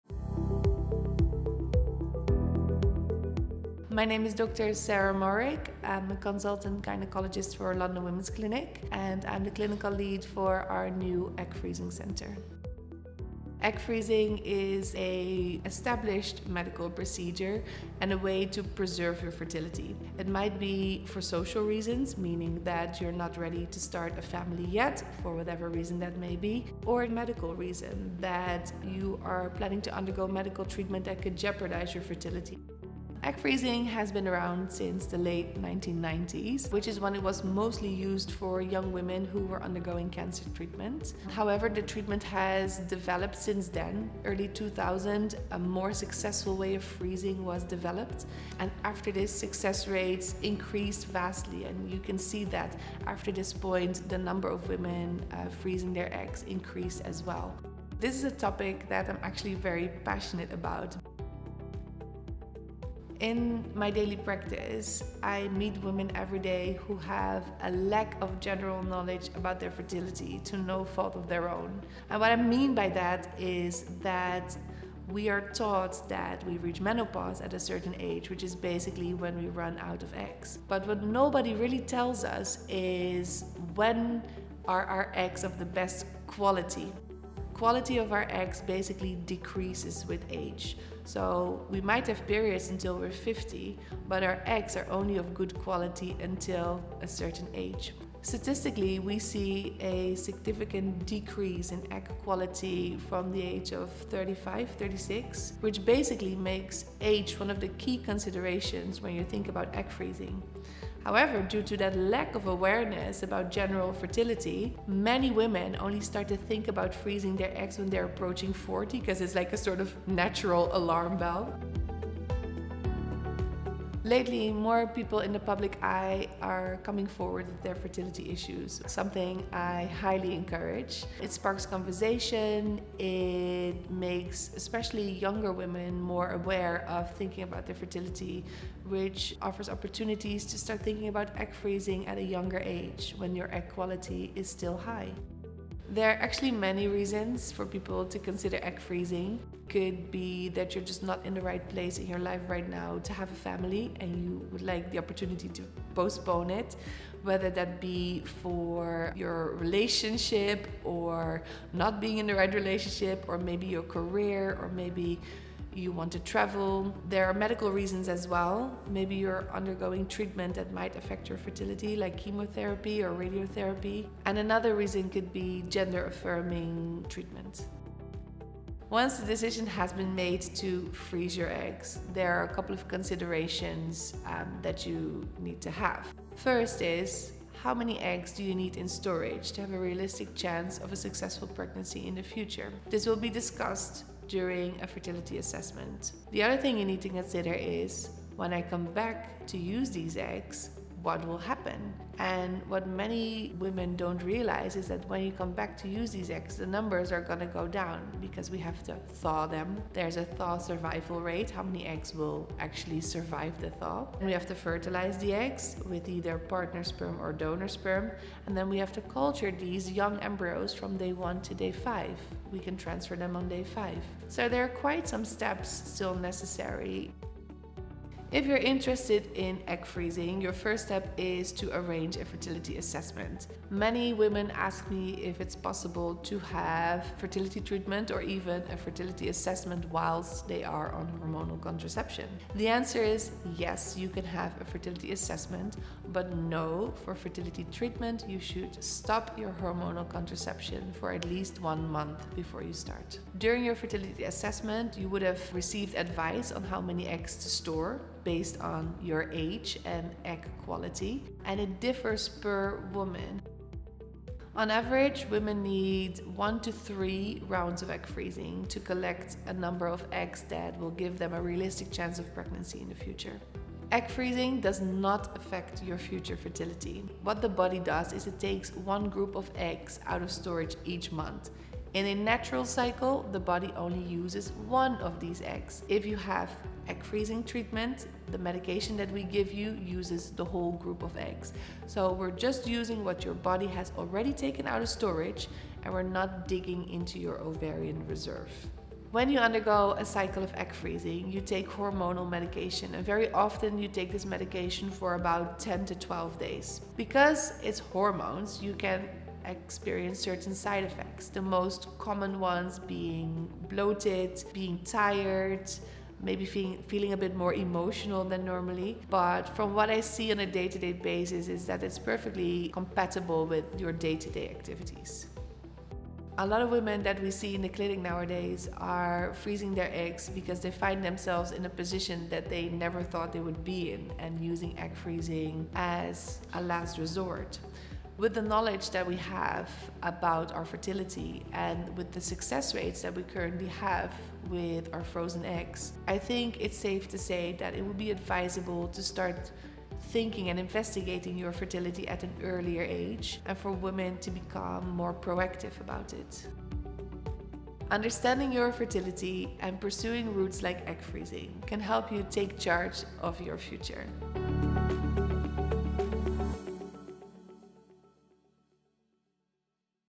Egg Freezing at London Women's Clinic
Clinical Lead for Egg Freezing at London Women's Clinic, Dr Sarah Mourik takes us through the advances in vitrification technologies that have enabled women to seize control of their own fertility